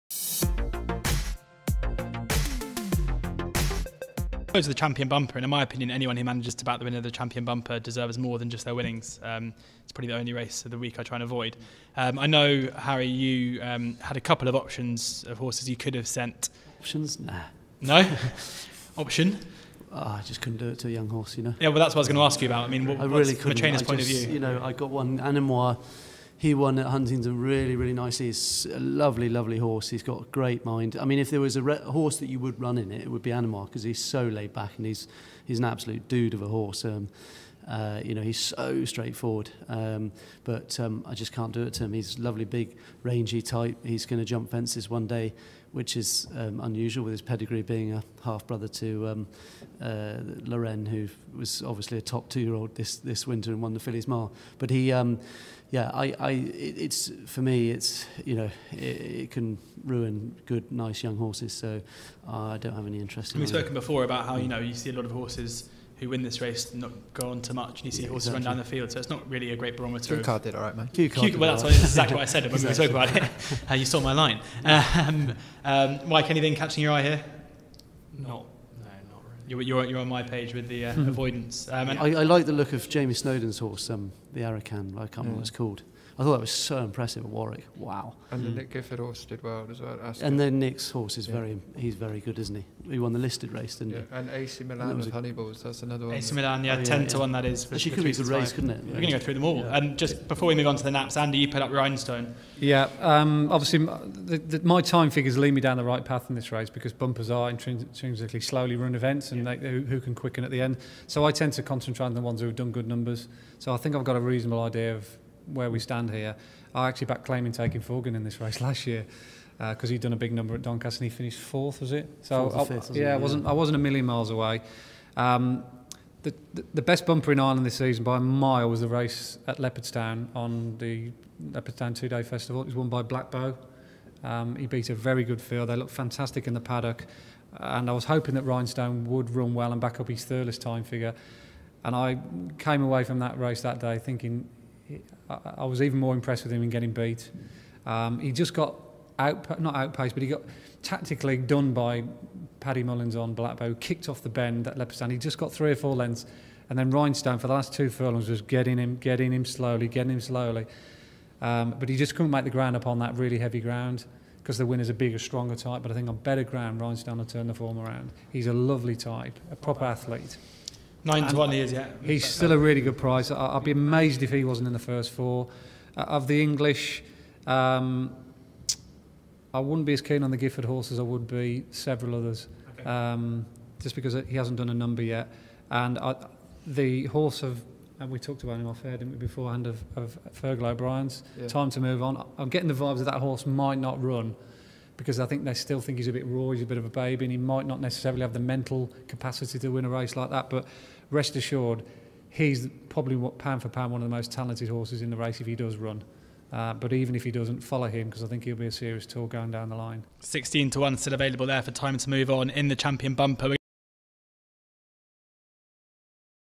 Wednesday 17:30 - Champion Bumper
Our expert Cheltenham Festival panel take us through the best bets for the race.

George Elek - Host
Andy Holding - Oddschecker Tipster
Nick Scholfield - Oddschecker Ambassador and Jockey
Harry Whittington - Trainer of Saint Calvados
Mike Spence - Pro Punter